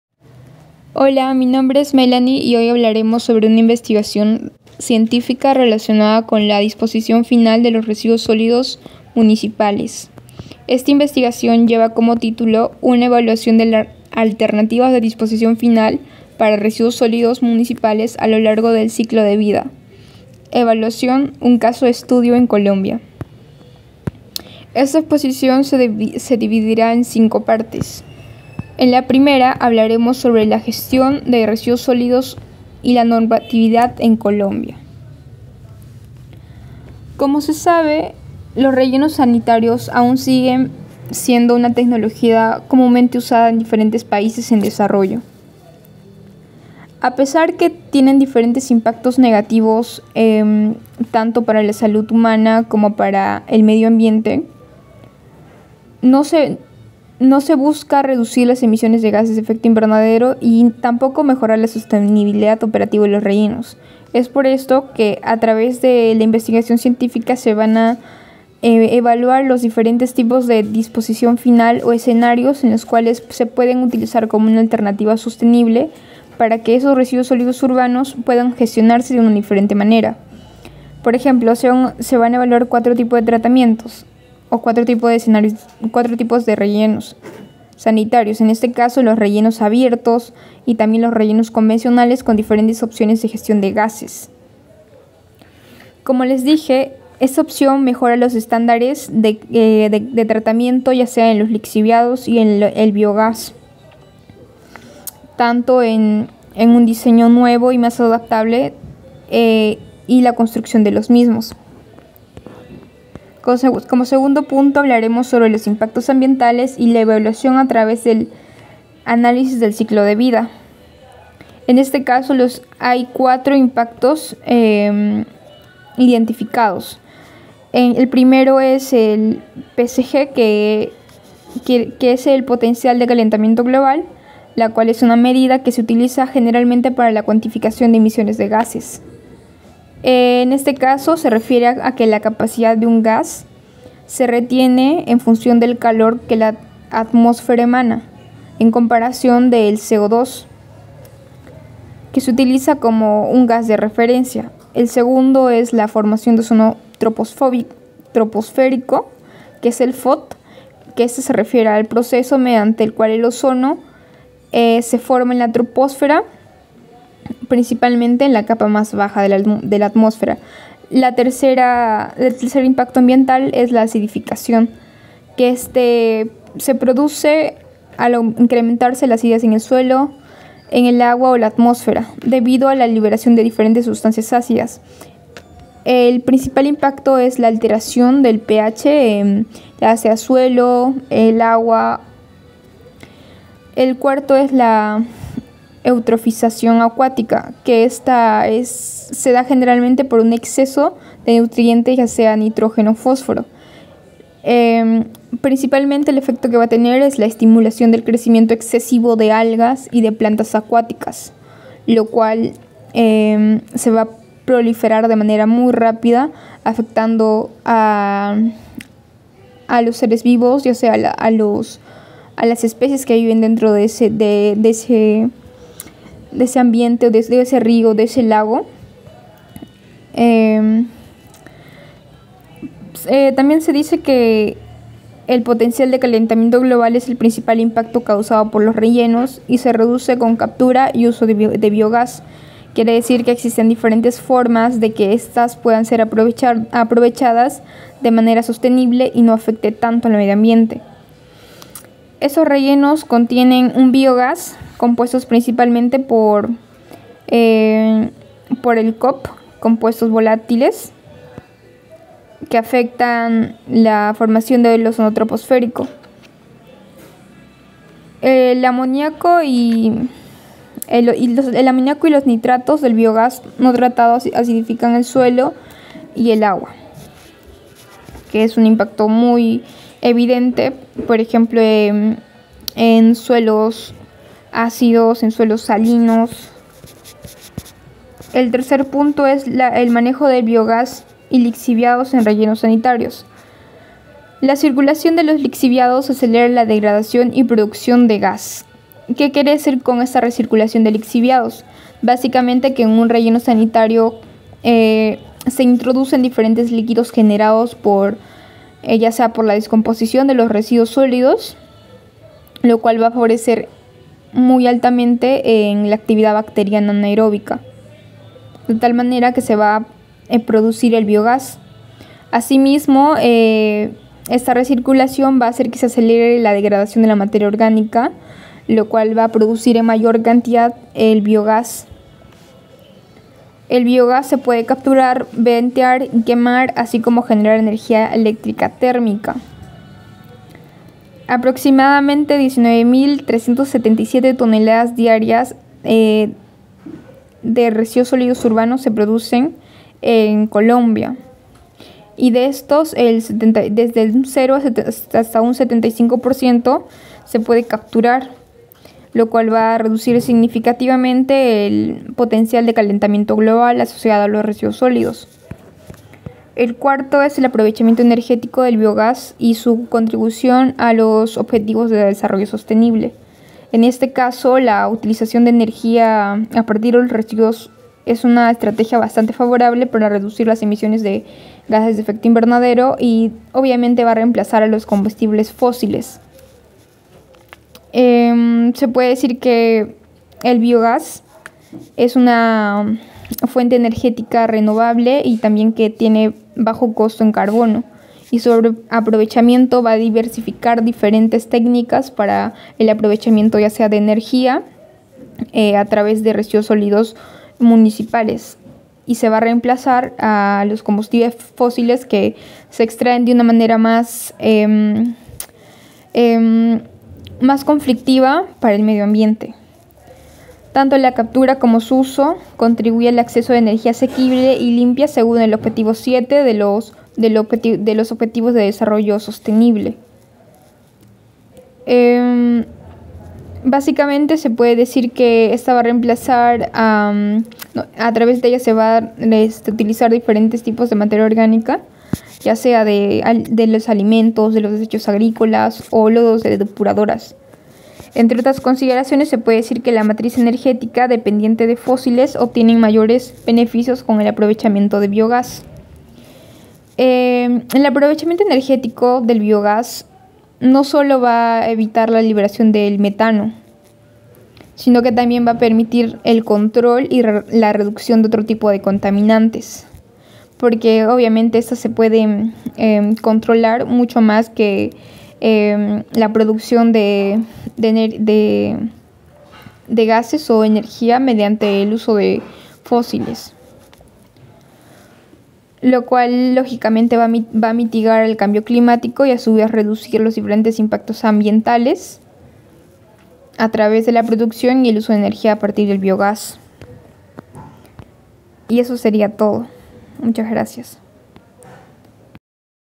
Evaluación de alternativas de disposición final para residuos sólidos a lo largo del ciclo de vida
Seminario del grupo:
Nina Condori Patrick Christian
Jeri Guzman Melany Maricielo